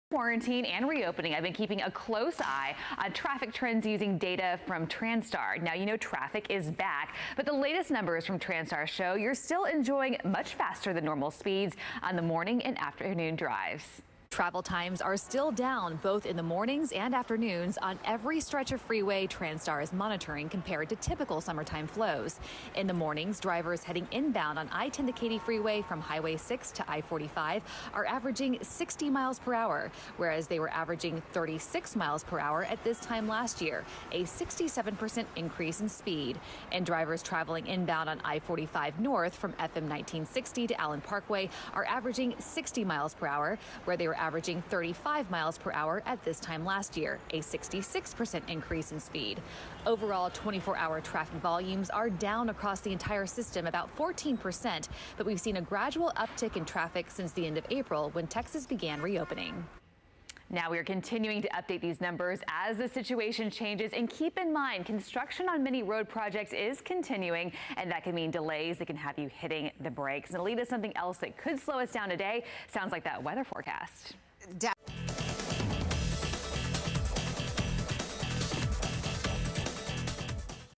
Traffic speeds still below average despite reopening
How has your commute been? Transtar says traffic across the Houston-area is still down 14% despite the gradual return of drivers to the roads.